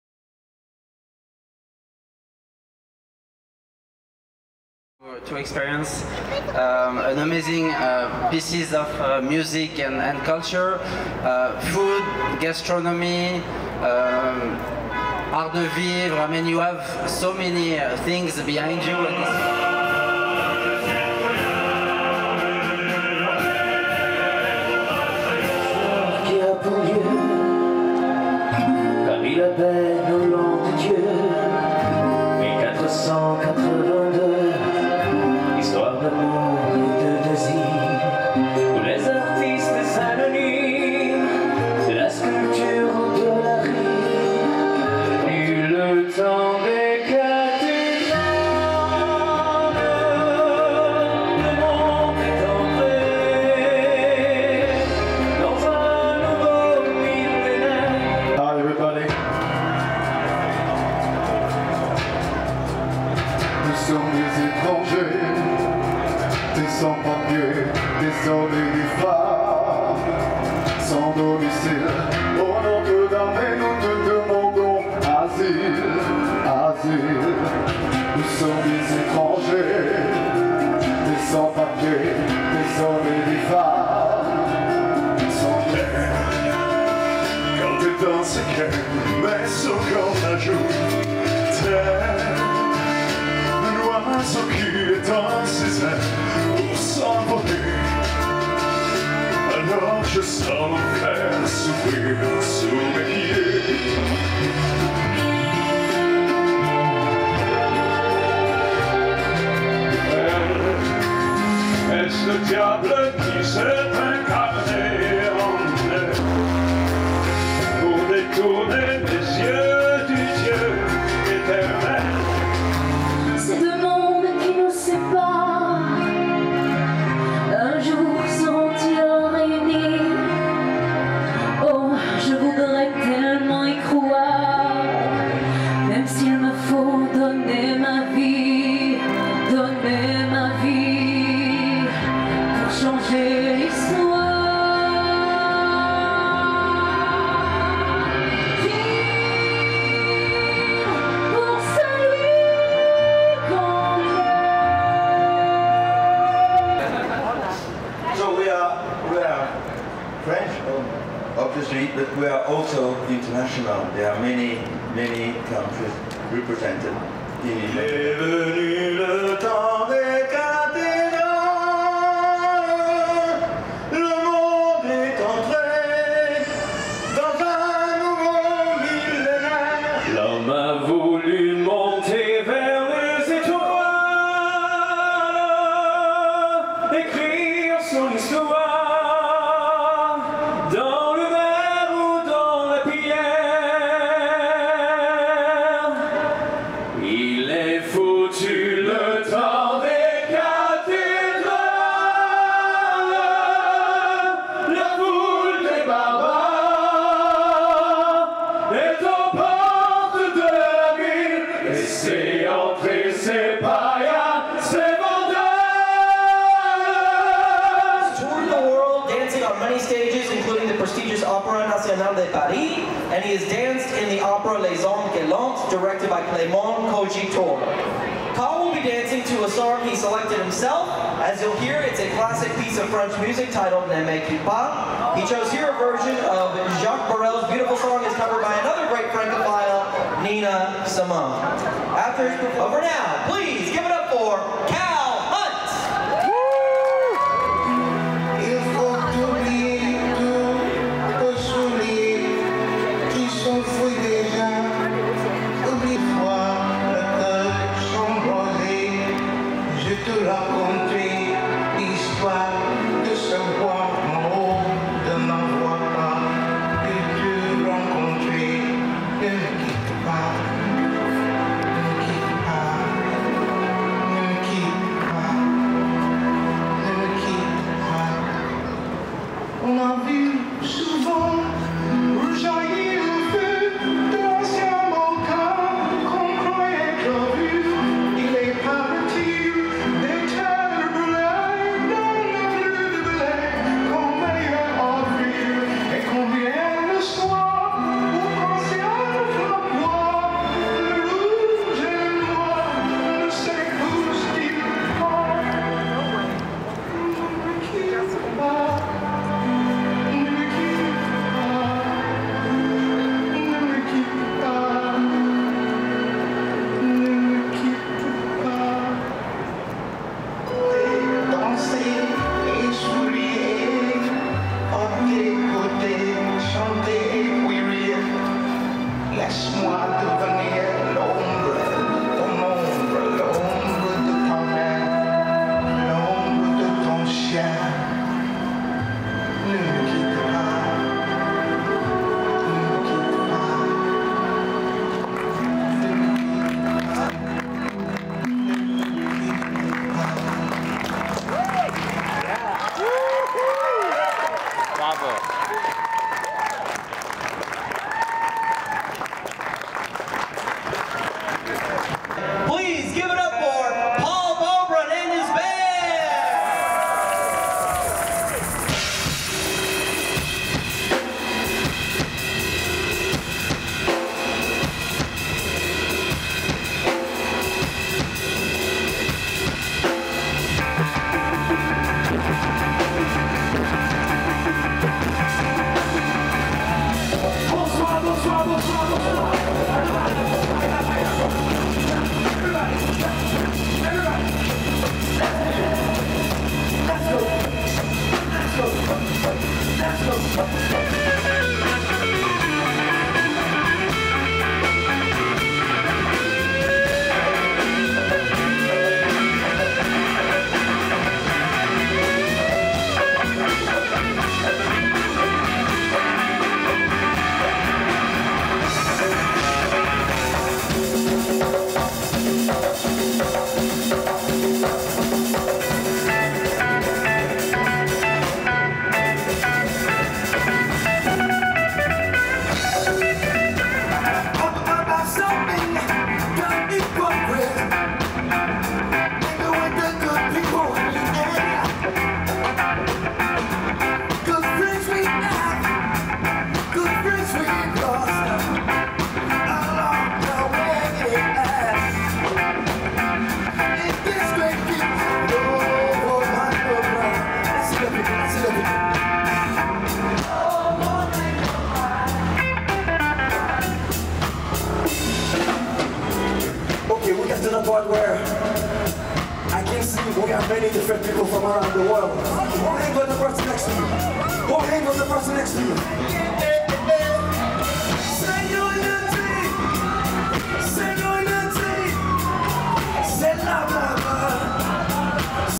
FIAF's Bastille Day Celebration NYC 2022 - Performances
FIAF’s popular Bastille Day celebration is back in person, happening for the first time on New York’s famed Madison Avenue!

Abounding with French cuisine, musique, dance, and myriad attractions for the whole family, this fête populaire invites New Yorkers to revel in the flavors, sounds, and joie de vivre of France.

For over 20 years, FIAF has hosted the city’s largest celebration of France’s national holiday, Bastille Day on 60th Street. This year’s event will expand to Madison Avenue and include live music and dance performances, wine and champagne tastings, a sneak preview screening of Azuro, fun activities in French for kids and adults, a chance to win a dream getaway to France’s Occitanie region and other exciting prizes, and more than 40 booths showcasing the best of French food and Francophone culture.